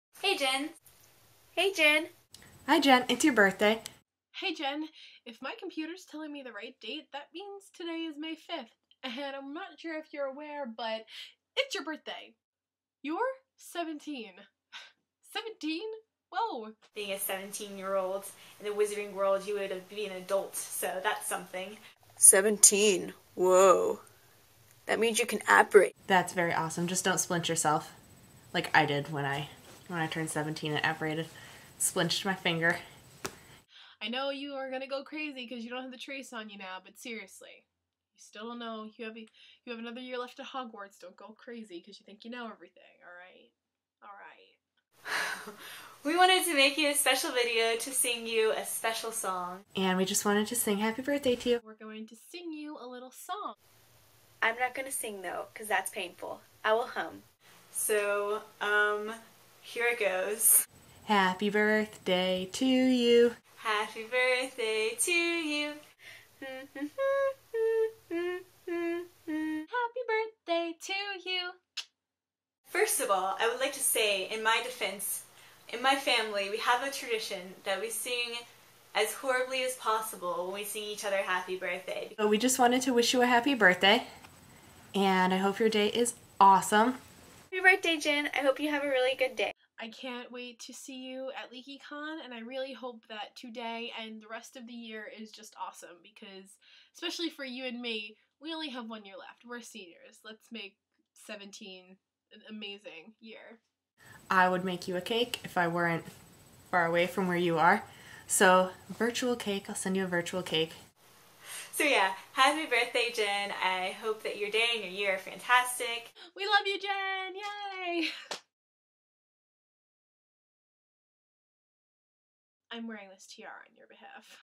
Jenn's Birthday Video!
This is a birthday video for our beloved Jenn! Happy birthday from Accio Office Fire :D

-Chelsea, Leah, Alicia, & Sarah :D